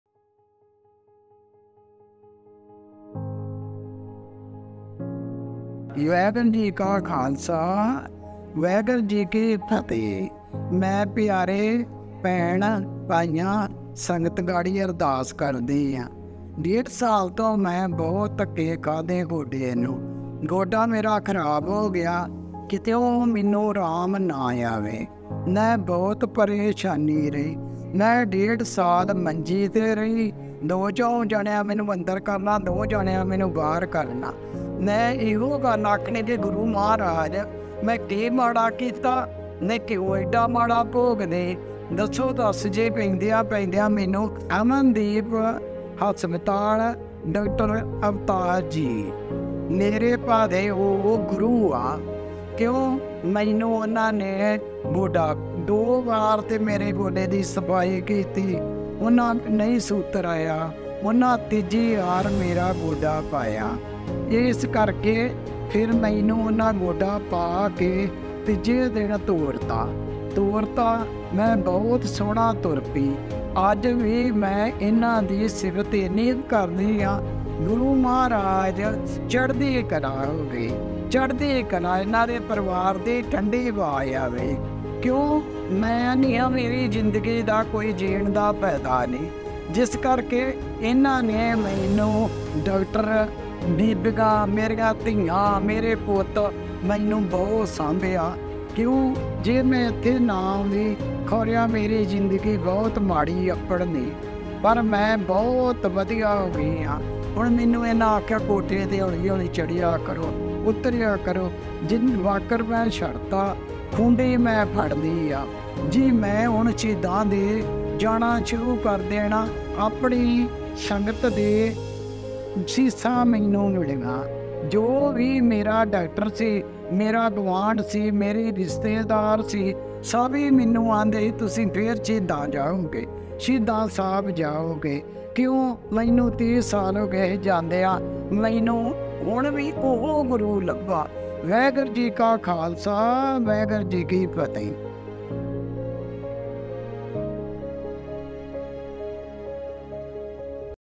A Patient's successful Tale of Robotic Knee Replacement by Dr. Avtar Singh at Amandeep Hospital
🌟 Experience the incredible transformation of one patient's life in "A Patient's successful Tale of Robotic Knee Replacement by Dr. Avtar Singh at Amandeep Hospital" 🌟

Join us on an inspiring journey as we share the heartwarming story of patient's  who went from suffering debilitating knee pain to reclaiming their freedom and mobility, all thanks to the cutting-edge technology and expert care provided by renowned orthopedic surgeon, Dr. Avtar Singh.

In this video, you'll witness:

👣 The patient's emotional journey, from the initial struggles to the triumphant recovery.
🤖 An in-depth look at the revolutionary robotic knee replacement procedure performed by Dr. Avtar Singh.
👨‍⚕️ Expert insights from Dr. Avtar Singh himself, shedding light on the advanced techniques that made this transformation possible.

This remarkable tale serves as a beacon of hope for anyone facing knee issues, showcasing the power of modern medicine and the dedication of healthcare professionals like Dr. Avtar Singh.

If you or a loved one are dealing with knee pain or considering knee replacement surgery, this video is a must-watch. Discover the possibilities of a pain-free, active life and learn more about the groundbreaking work being done in the field of orthopedic surgery.

Don't miss this heartening story of resilience, healing, and the incredible partnership between patients and their healthcare providers.